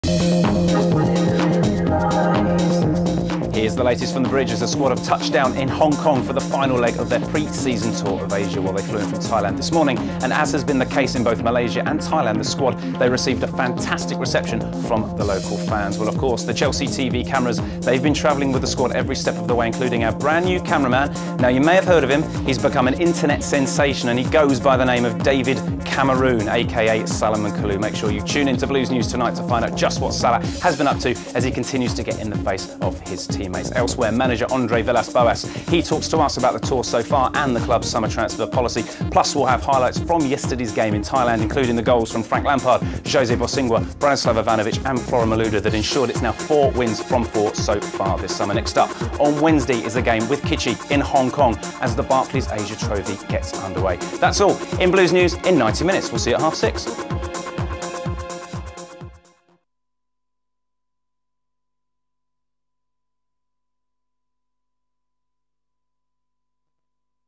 Chelsea FC - Blues news headlines
Lee Parker brings you all the latest news from Stamford Bridge.